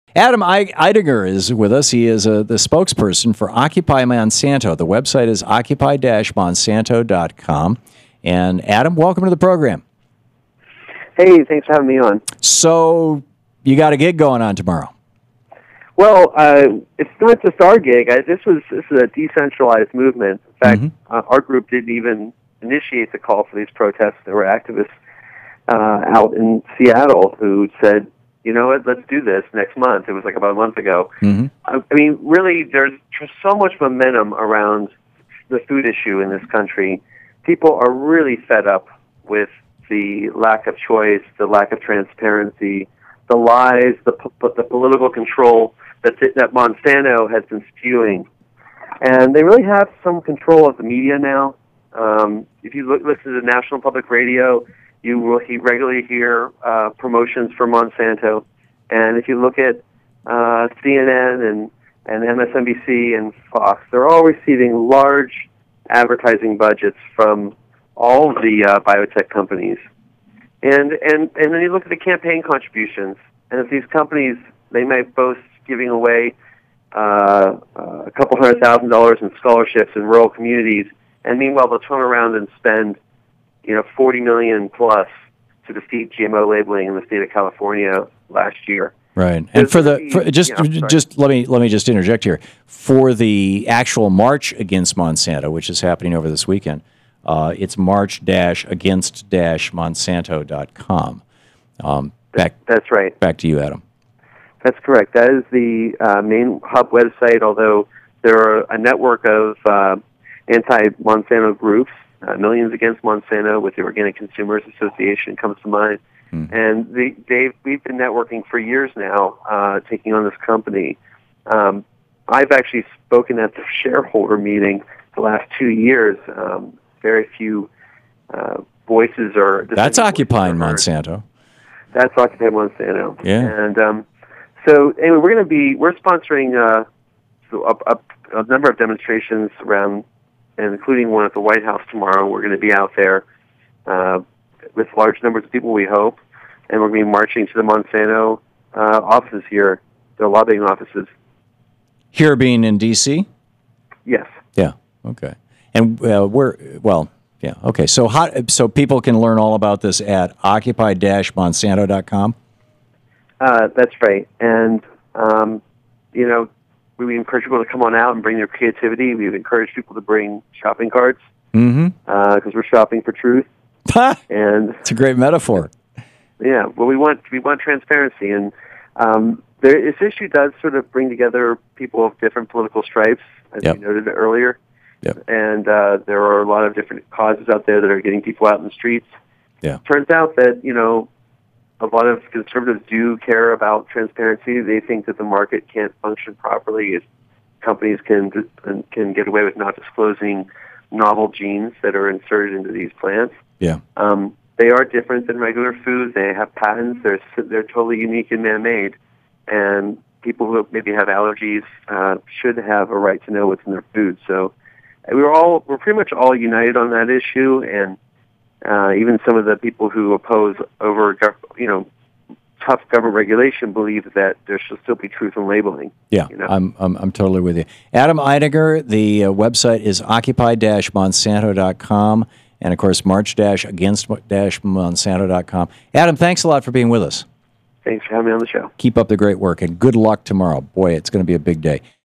Occupy Monsanto: Shopping for the Truth
Thom Hartmann talks with Adam Eidinger, spokesperson for Occupy Monsanto, about protests against Monsanto corporation slated for Saturday, may 25, 2013.